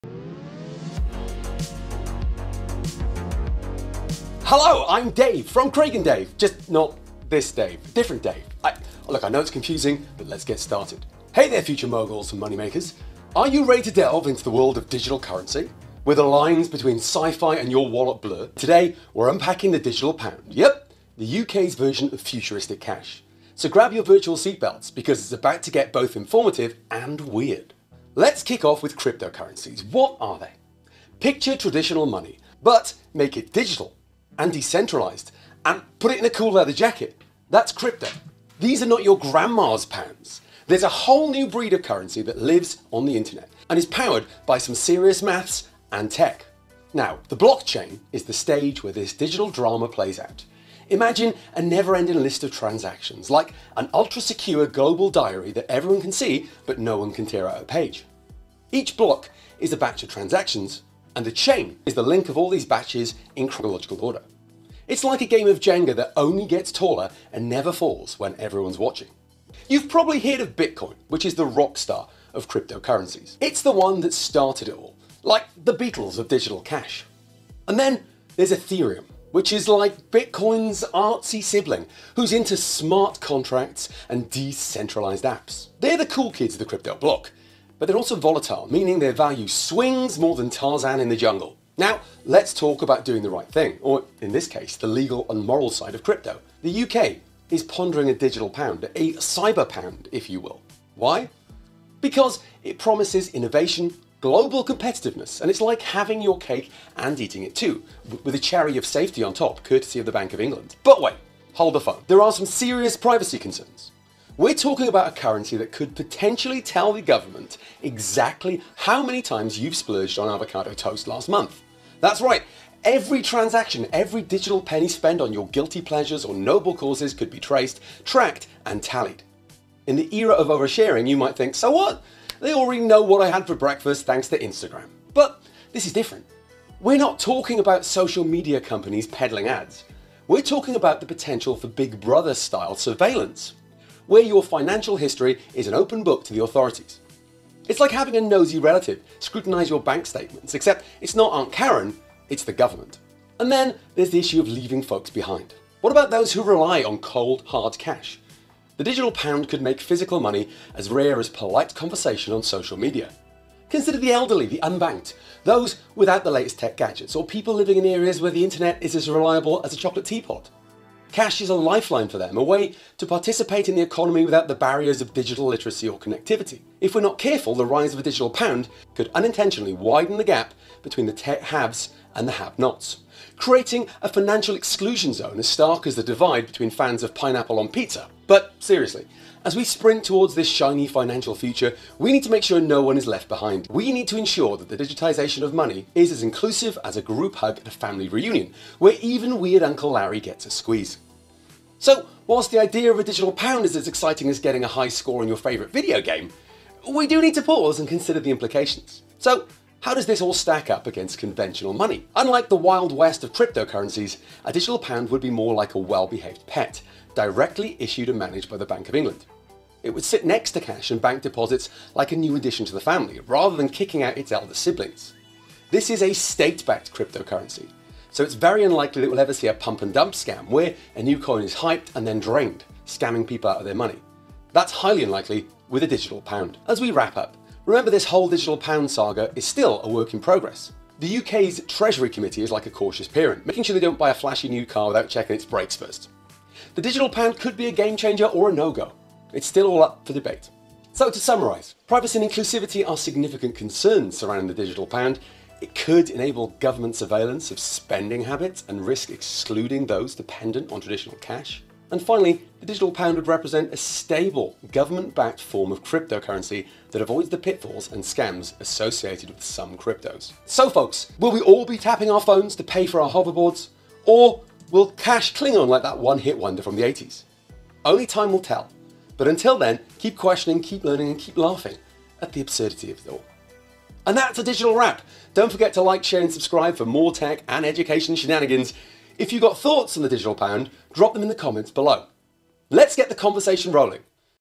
Digital Pound - Isn't that Bitcoin with more steps?
Delve into the world of digital currency where the lines between sci-fi and your wallet blur.

Fertile questions:
What are the problems with introducing a digital pound?
Will digital currencies eventually replace existing currencies?
Will we ever be completely cashless?